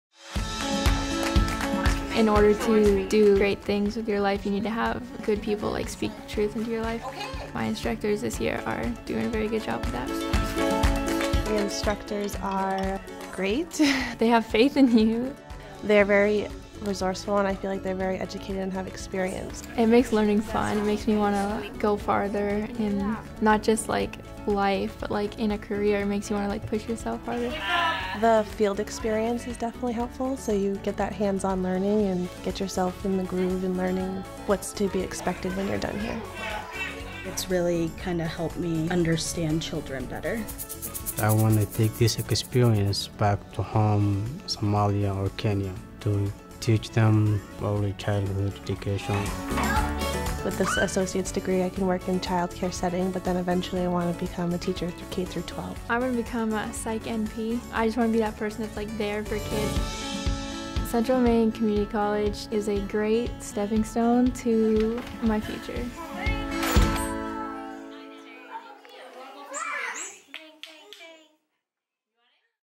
Early Childhood Education Degree at Central Maine Community College
The Early Childhood Education degree combines understanding and application of theory to practical experiences working directly with young children, newborn through preschool.

Learn more and download the program sheet with classes.